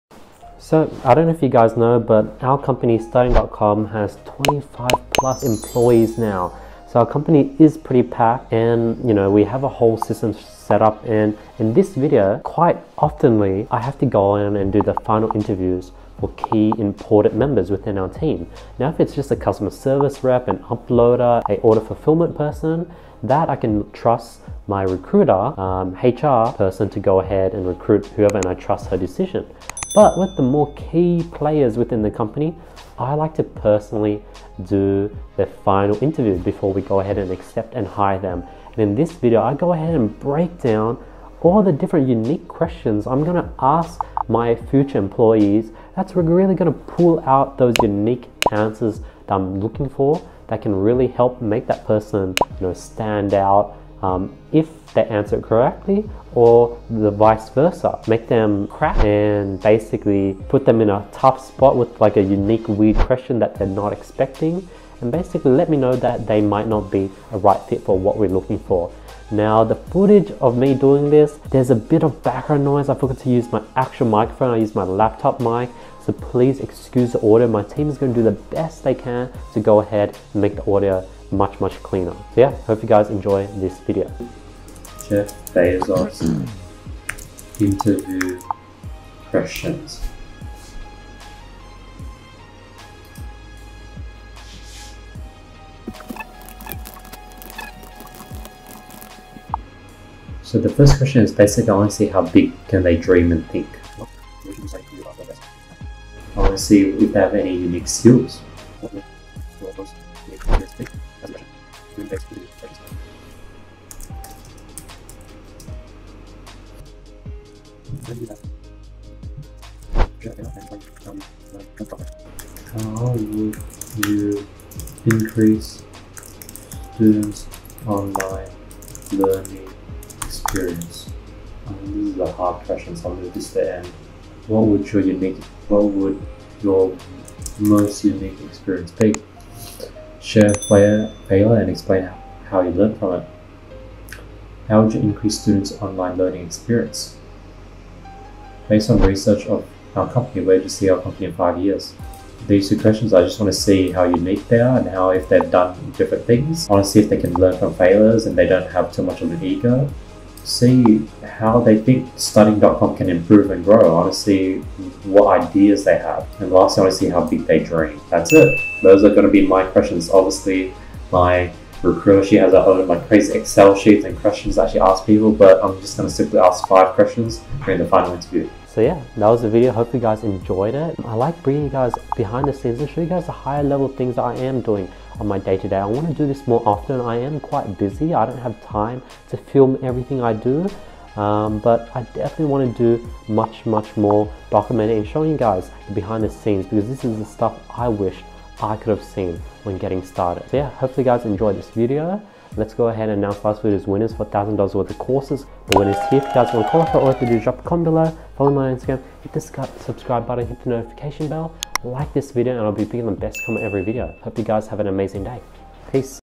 Coming Up With My Company's Interview Questions
As the saying goes, don't judge the book by its cover. You should know the book. You should read the book, before saying if it's good or bad. Same as employing people. You should know the people, you should be able to read who the real them, you should identify if they're right or not for your organization.

So how do I personally get to know my new employees? Watch this video today to know how I device my questions for new employees.

Answering ALL Questions, Mentorship Requests, Video Requests, Help
- @andyqmai (instagram)